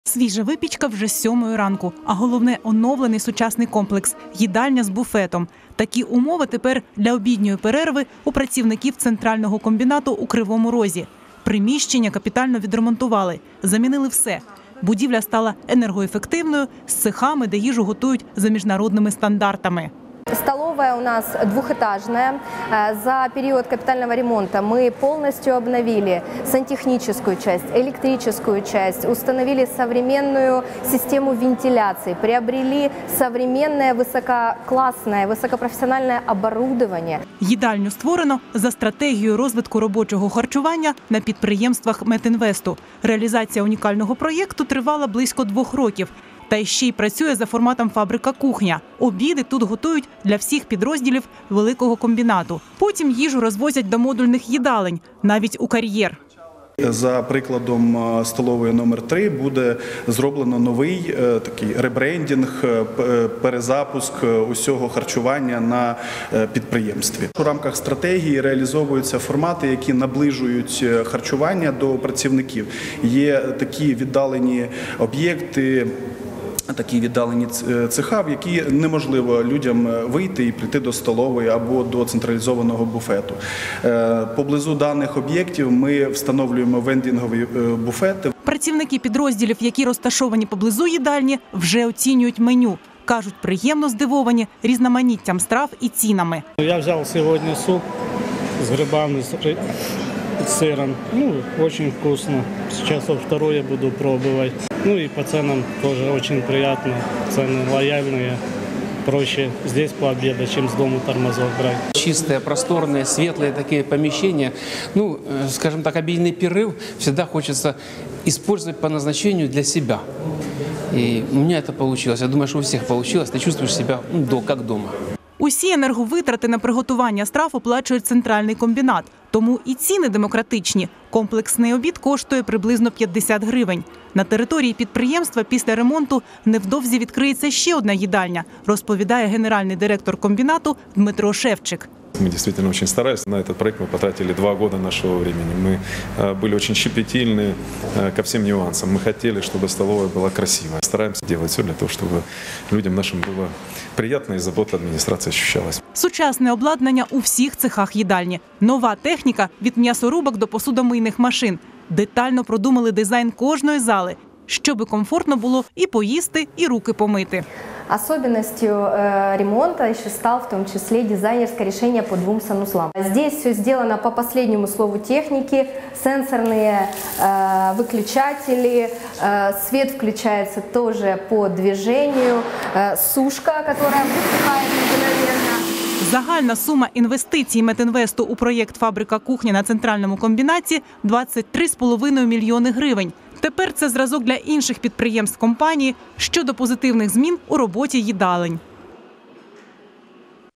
У Кривому Розі оновили їдальню Центрального комбінату
У Кривозі Розі на підприємстві Центрального ГЗК зробили капітальний ремонт Їдальні. Тепер там готують  їжу за міжнародними стандартами.

Головні новини Дніпра та регіону  на 34 телеканалі.